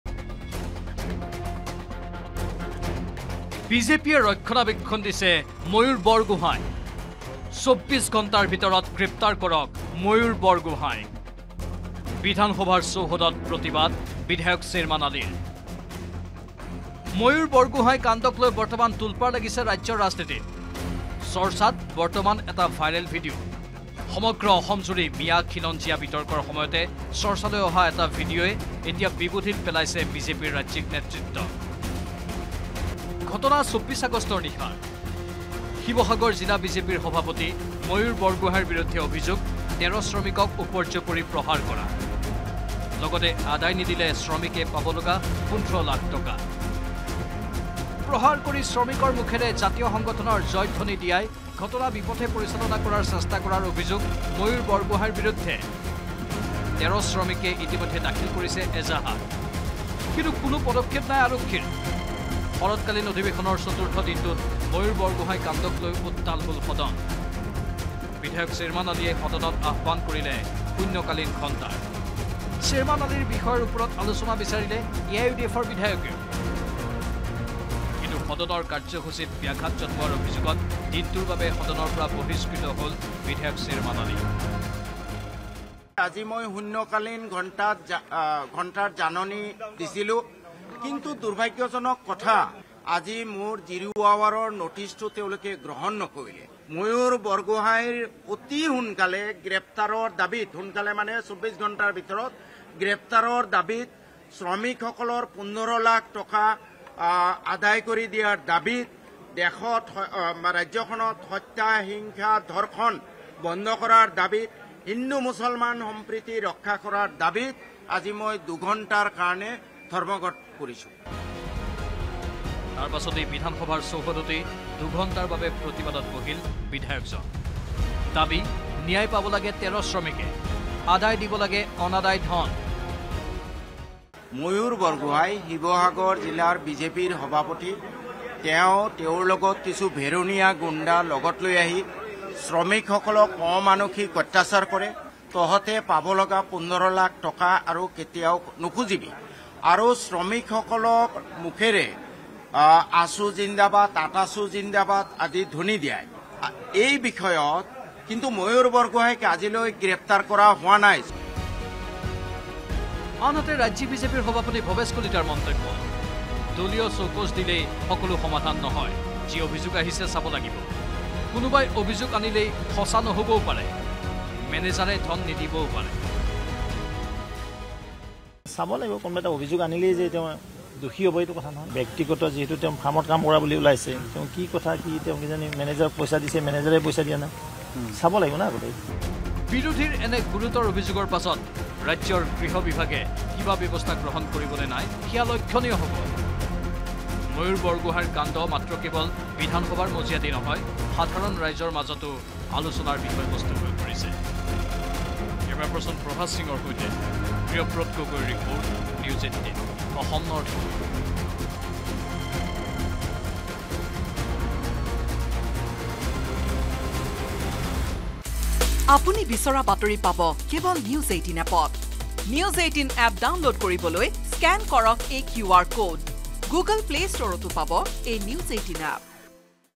Sherman Ali Protest News | বিধানসভা চৌহদত প্ৰতিবাদ বিধায়ক শ্বেৰমান আলীৰ N18V
বিধানসভা চৌহদত প্ৰতিবাদ বিধায়ক শ্বেৰমান আলীৰ
ময়ূৰ বৰগোহাঁইক গ্ৰেপ্তাৰৰ দাবীত প্ৰতিবাদ
বিজেপিয়ে ময়ূৰ বৰগোহাঁইক ৰক্ষণাবেক্ষণ দিয়াৰ অভিযোগ
শিৱসাগৰ জিলা বিজেপিৰ সভাপতি ময়ূৰ বৰগোহাঁই
প্ৰাপ্য নিদি ১৩ শ্ৰমিকক উপৰ্যুপৰি প্ৰহাৰ কৰাৰ অভিযোগ